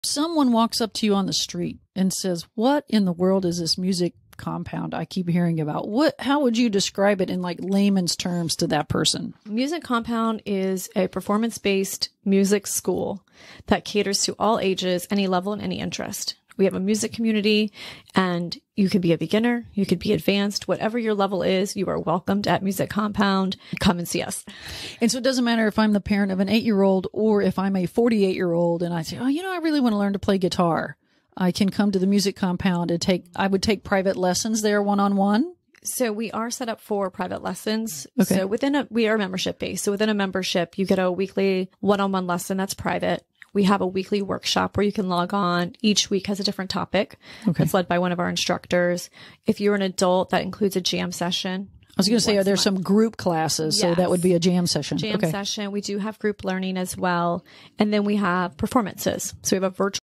Episode 63: Jenny Townsend
A fifth generation Sarasotan was working at the Packinghouse Cafe and Leroy Selmon's Restaurant when she noticed that area musicians needed a place to interact, engage, and learn from each other. After several years of research, a business plan was hatched and Jenny Townsend is now in her sixth year of running the Music Compound, a performance-based music school that caters to all ages, any level, and any interest.
Hear her story of entrepreneurship, perseverance, a year of mask making, and all the awesome offerings the Music Compound has for all musicians, young or old!
or on any podcast player.
Come along and join the club!